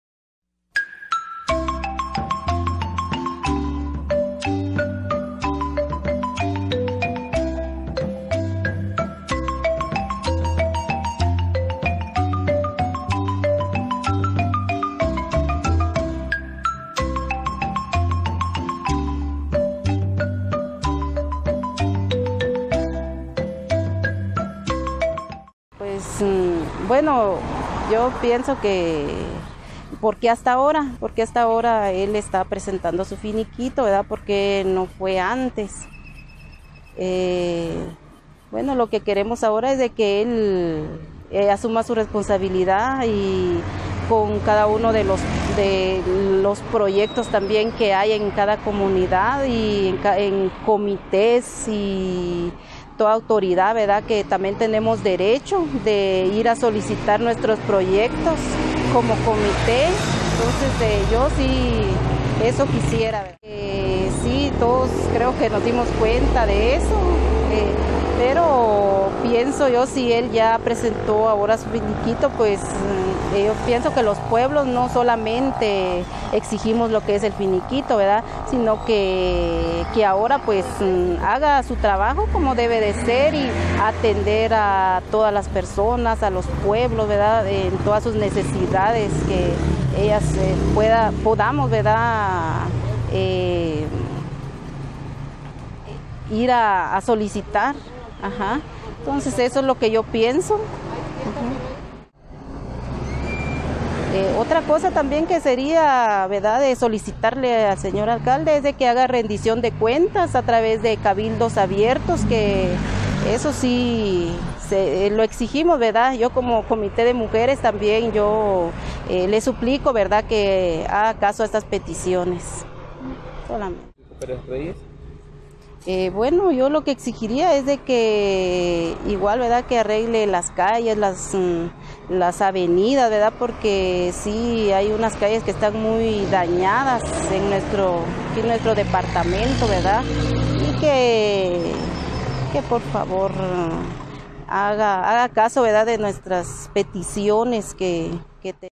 Vecinos de Santa Cruz, Quiché, solicitan al alcalde que los proyectos continuen en cada comunidad
Se le exige que haga bien su trabajo y atender las necesidades de cada comunidad así como una rendición de cuentas en cabildo abierto, "como comité de mujeres de Santa Cruz se le solicita que haga caso a las peticiones planteadas".